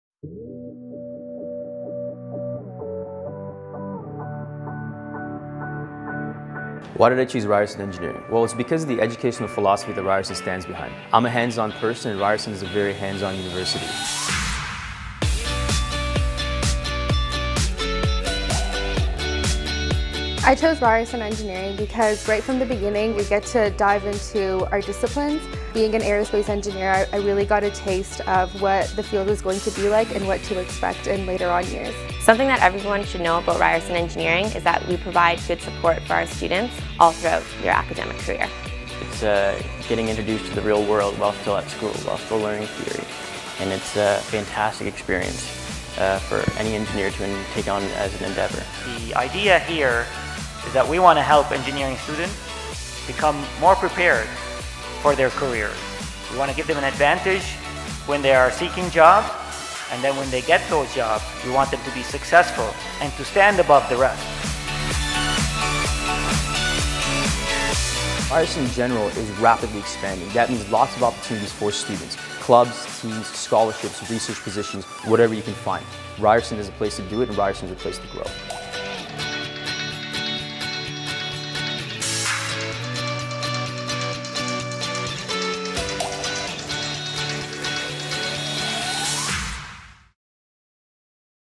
Toronto Metropolitan Engineering
Toronto Metropolitan Engineering students are catalysts for change. Armed with a Toronto Metropolitan education and their own creativity, they solve problems and transform our world. Toronto Metropolitan Engineering students are leaders of tomorrow. They are entrepreneurs. They are difference-makers. We hope that you'll be one of them.